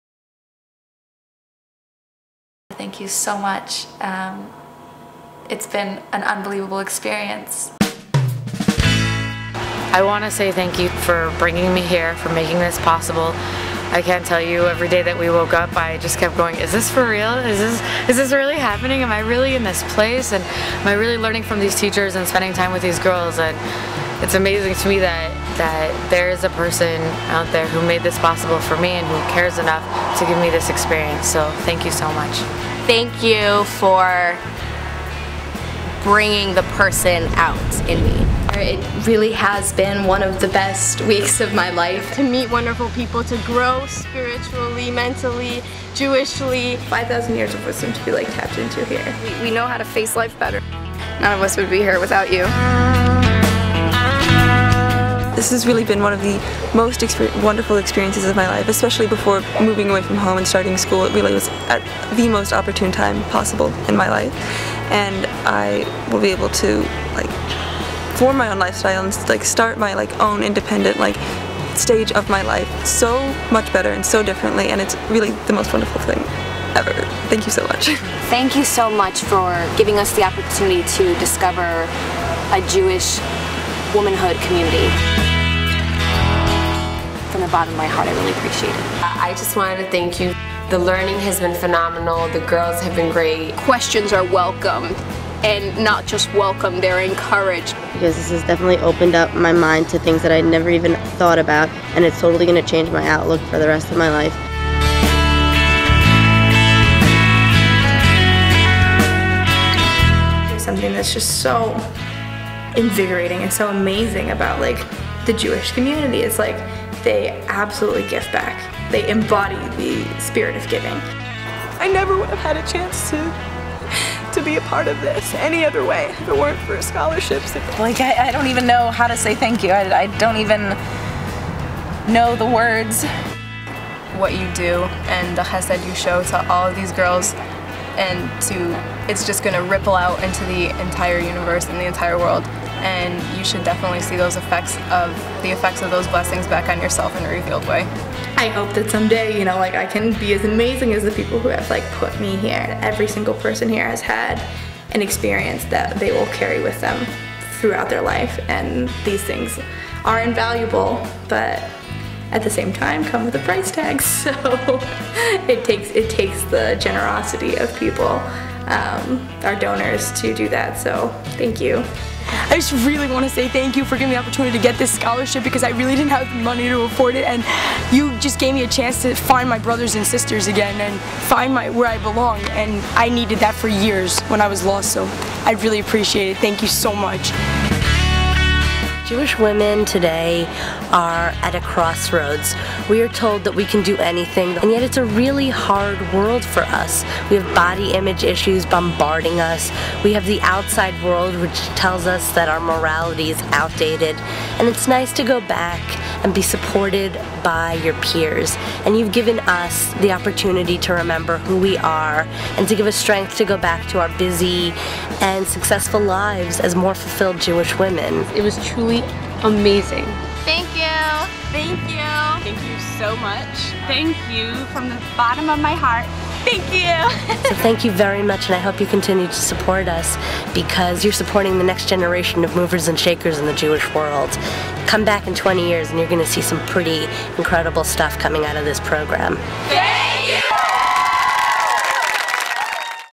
Snorkel & Study Thank Yous.mov
Snorkel & Study participants say their heartfelt thank yous